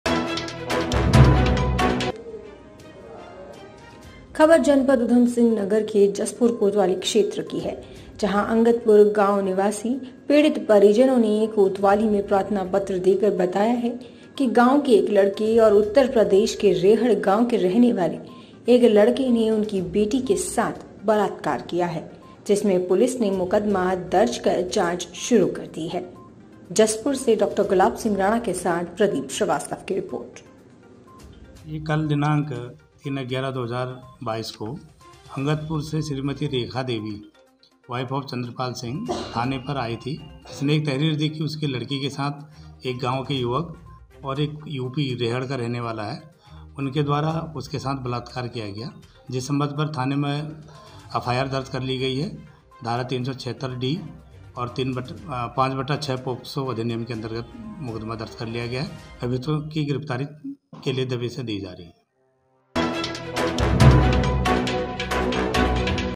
जसपुर : बलात्कार में मुकदमा दर्ज
स्लग- बलात्कार में मुकदमा दर्ज
रिपोर्टर- डॉ गुलाब सिंह राणा के साथ प्रदीप श्रीवास्तव की रिपोर्ट
स्थान - जसपुर

एंकर- ख़बर जनपद उधम सिंह नगर के जसपुर कोतवाली क्षेत्र की है जंहा अंगदपुर  गांव निवासी पीड़ित परिजनों ने कोतवाली में प्रार्थनापत्र देकर बताया कि गांव के एक लड़के और उत्तरप्रदेश के रेहड़ गांव के  रहने वाले एक लड़के ने उनकी बेटी के साथ बलात्कार किया है जिसमे पुलिस ने मुकदमा दर्ज कर जांच शुरू करदी है

वी ओ - मामला जसपुर कोतवाली क्षेत्र के गांव अंगदपुर का है जंहा रेखा देवी ने तहरीर देकर बताया कि उसकी नाबालिग बेटी के साथ दो युवको ने बलात्कार किया है जिसमे पुलिस ने मुकदमा दर्ज कर लिया है वंही कोतवाली जसपुर एस आई हरीश आर्य ने बताया कि अंगदपुर गांव से रेखा देवी द्वारा एक प्रार्थनापत्र दिया गया था जिसमे गांव के ही रहने वाले एक युवक ओर उत्तरप्रदेश के रेहड़ गांव के रहने वाले एक युवक ने उनकी नाबालिग बेटी  के साथ बलात्कार किया है जिसमे पुलिस द्वारा 376D ओर पॉक्सो अधिनियम के तहत मुकदमा दर्ज कर लिया गया है ओर अभियुक्तों की गिरफ्तारी के लिए दविश दी जा रही है